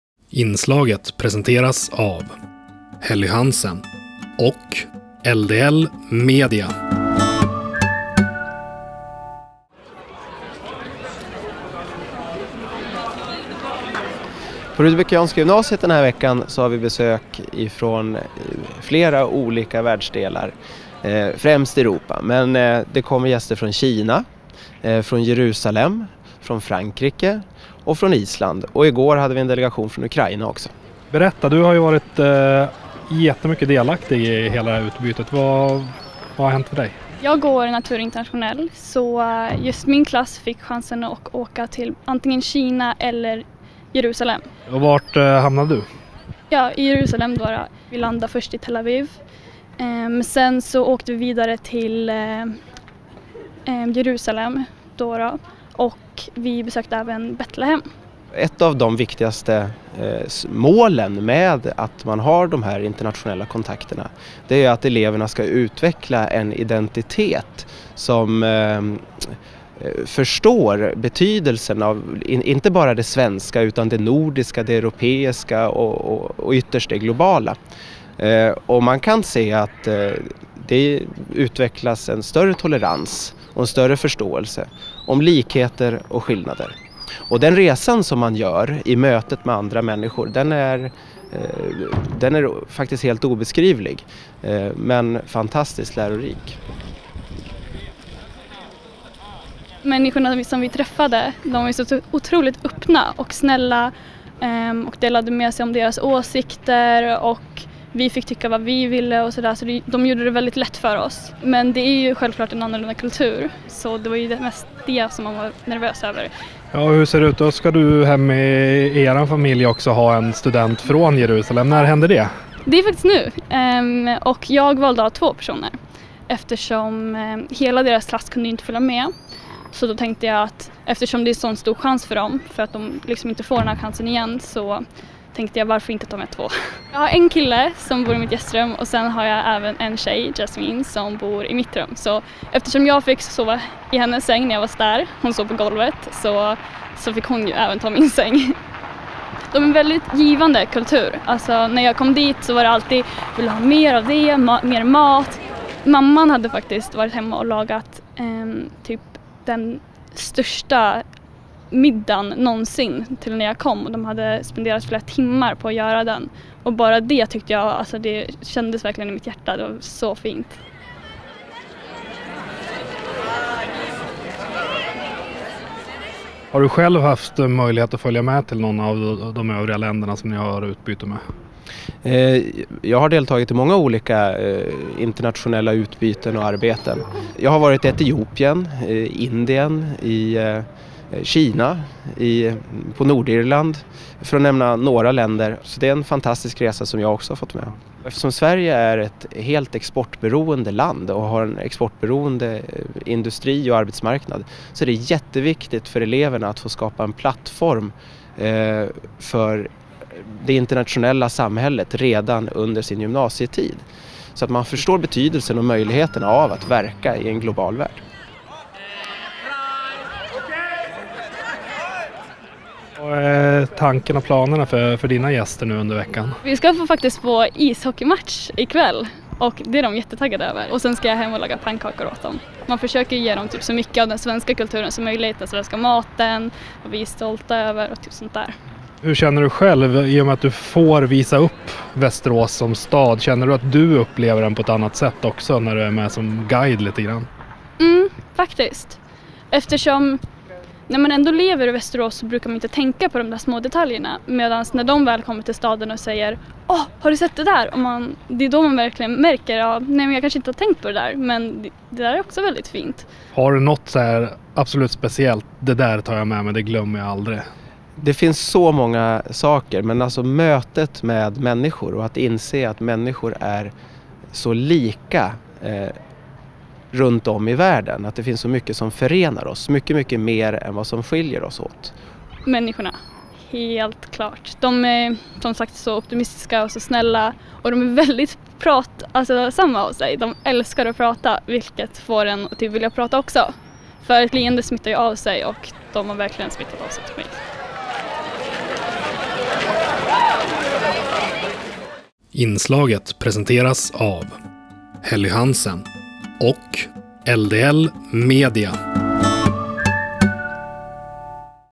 Internationella besök på Rudbeckianska gymnasiet
I veckan har det varit stor internationell närvaro på Rudbeckianska gymnasiet i Västerås. Vi har pratat med David Borg som berättar om detta samarbete och Fanny Holmqvist som delar med sig av hur det är att hälsa på i andra kulturer och även få hem nya vänner till sitt eget hus.